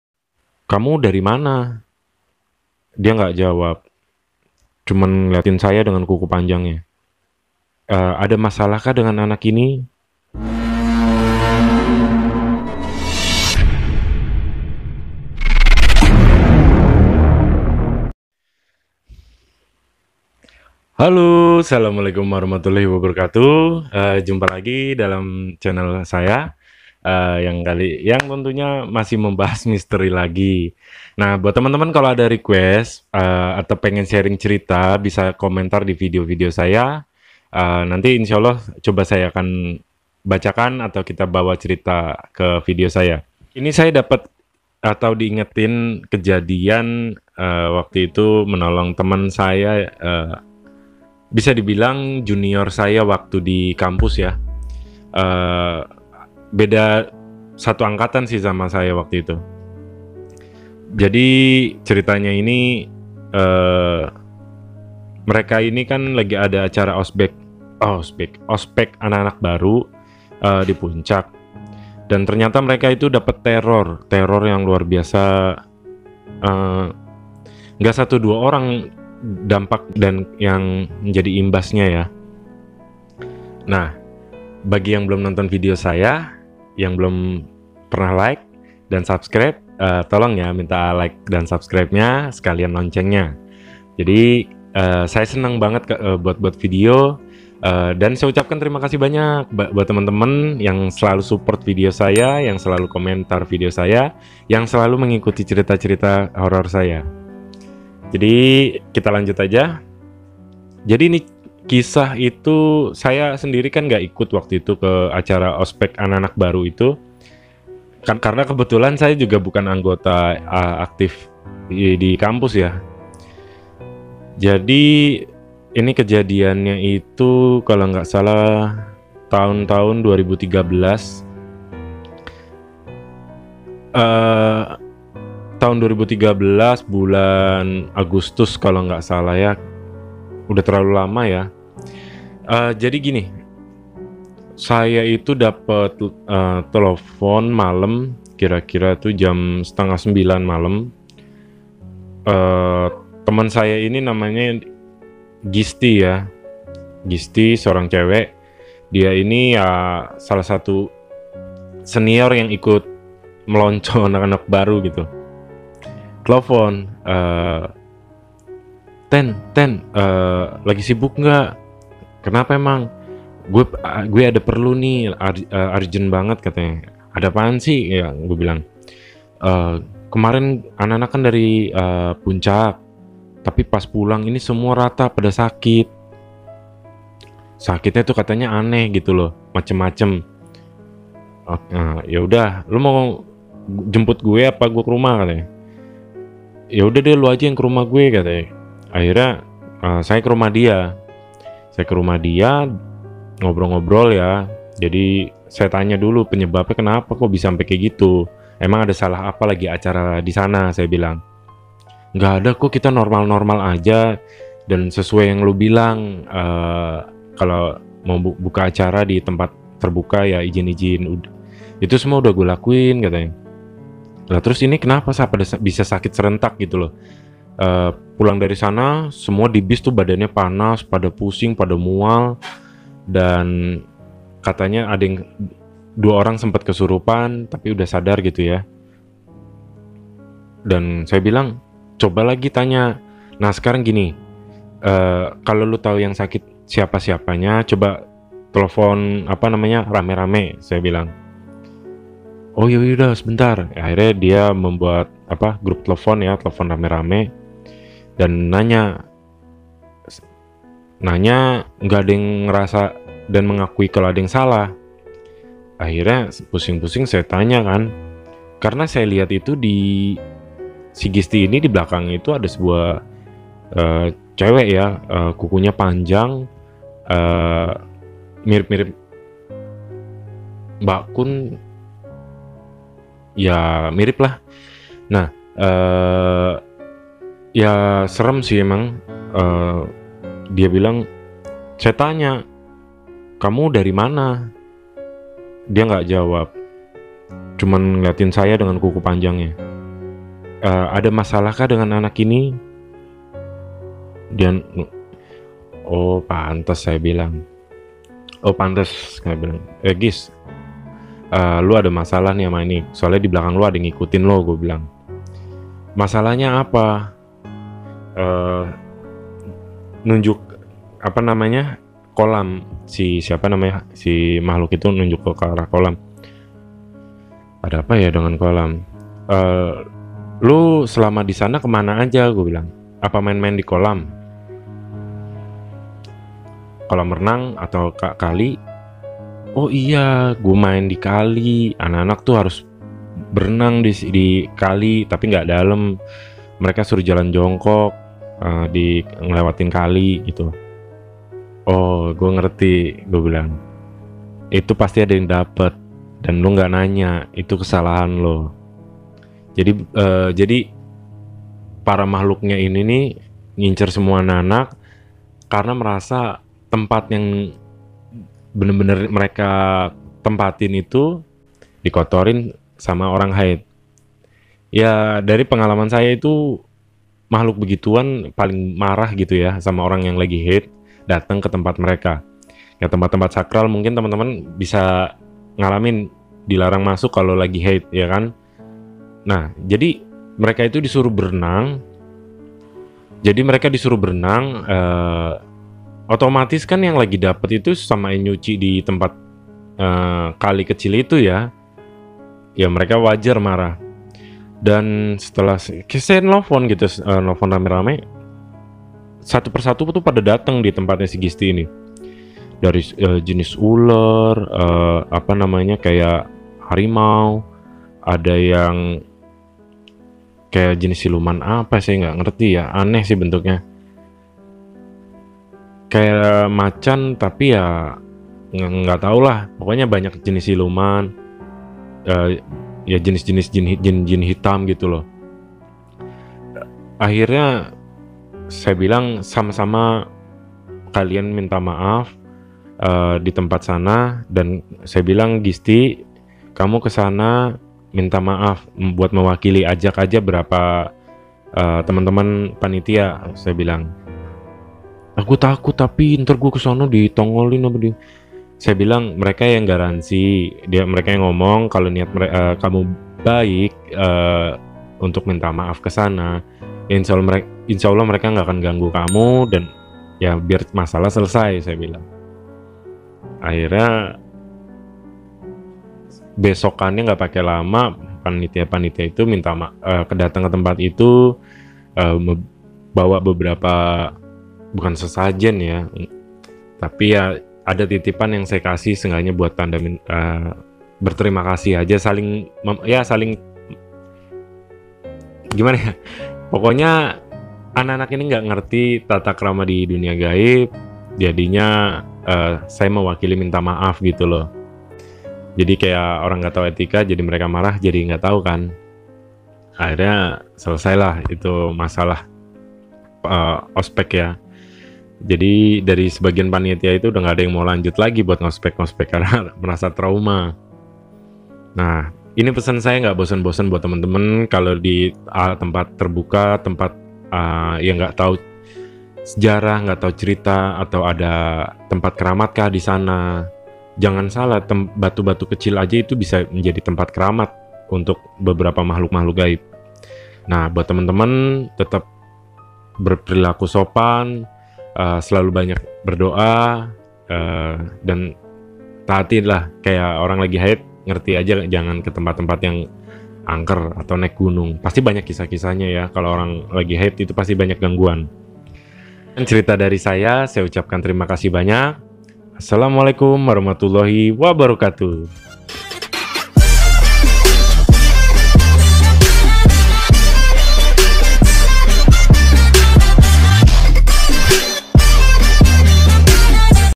Teror hantu Seram seusai ospek | Cerita hantu kisah nyata terbaru
cerita horor yang berdasarkan kisah nyata yang di alami teman kampus saya dulu kisah pengalaman ganjil mistis ini terjadi ketidak Tahuan teman saya dalam beretika di alam kisah pengalaman yang mendebarkan dan menakutkan ini saya ceritakan berdasarkan kisah nyata

bagi teman teman yang ingin berbagi cerita mistis atau cerita hantu silahkan komentar ya pada video
Cerita hantu kisah nyata - Teror hantu Seram seusai ospek kampus